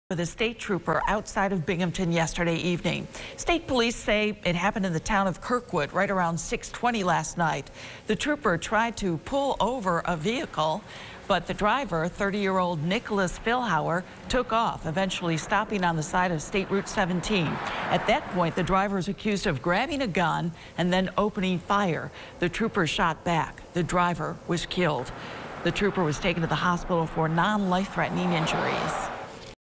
Driver dies after shootout with troopers
The trooper sustained non-life threatening injuries.